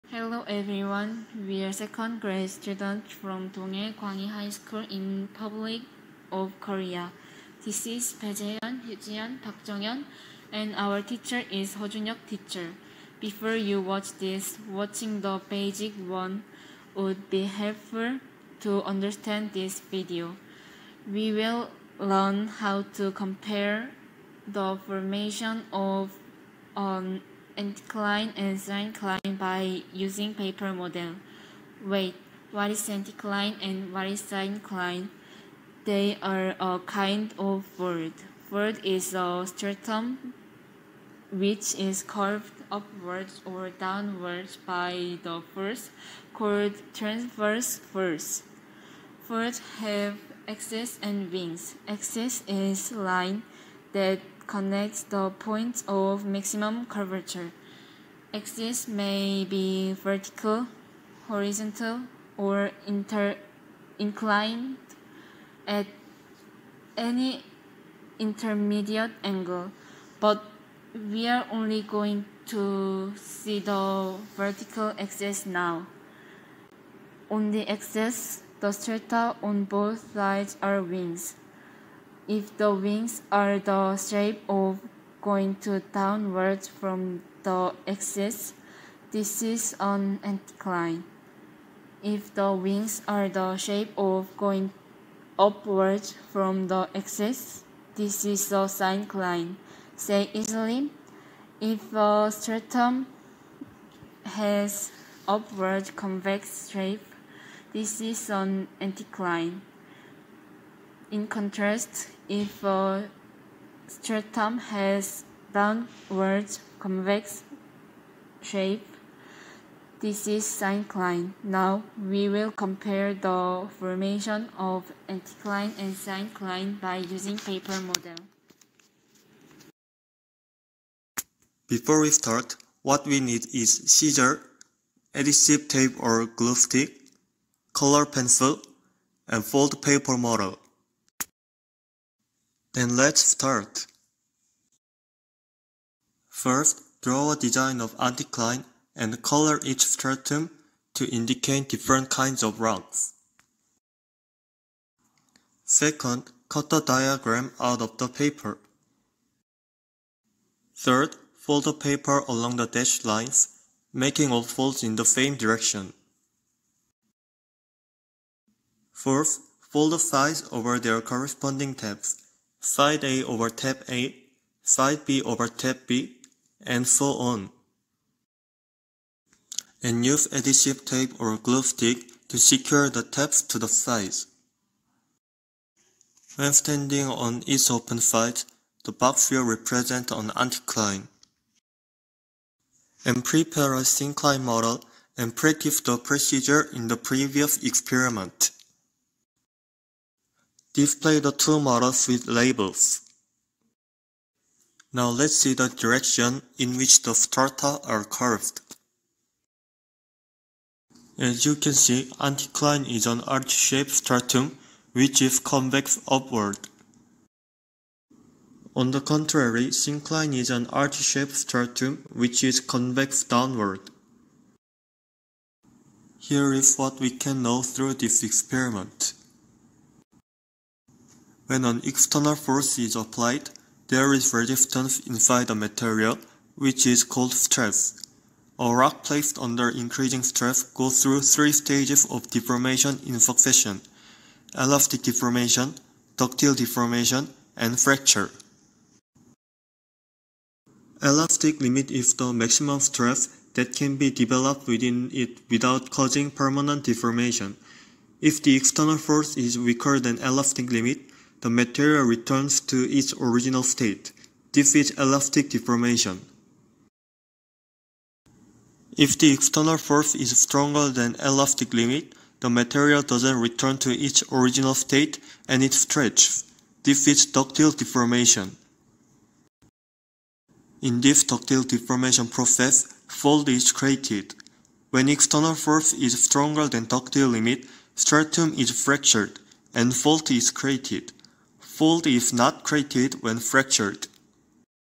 Crustal Bending: Deformation of the Earth's Crust-Advanced(Science Festival 2022 in Osaka)
■ Title: Crustal Bending: Deformation of the Earth’s Crust-Advanced
■ Main Lecture: Jun-Hyuk Heo
■ Sub Lecture: Jae-Hyeon Bae, Ji-Yeon Yoo, Jeong-Hyeon Park
■ Belong to: Dong Hae KwangHee High School in Republic of Korea.

Hellow~^^
I am Jun-Hyuk who have been participating in Science Festival in Osaka since 2013.
I am a Science teacher from Dong Hae Kwang Hee High School in Republic of Korea.
My Students made the two types of video of science experience activity for Science Festival 2022 in Osaka for themselves through my teaching.
One is basic experience activity and anther is advanced experience acitivity for Crustal Bending in the Earth.
This video is the advanced activity for understanding about crustal bending.
This video’s runtime is 4minutes and 54 seconds.
The aims of this lesson is comparing of the formation of an anticline and syncline using paper model.
For this activity, You have to prepare the glue stick or adhesive tape, Scissors, Fold paper model(anticline and syncline) such as Figure 2, Figure 3.
About At 3 minutes and 31 seconds of the video, you can see the completed model of the syncline and the anticline.
Stop playing the video for a moment and Compare the two model's shape.
You can see the same rocks and strata in Both of two upper side that you had made already.(The same color and pattern are the same kind of rock and stratum.) But You can see the different shape of rock and strata in front side. Left is Syncline structure and Right is Anticline structure in normal fold.
Let's look at the description of the video and follow the science experience activity about crustal bending: Deformation of the Earth's Crust-Advanced.
I hope everyone who does this hands-on activity has a good time.
I want to wish you all a happy and good health~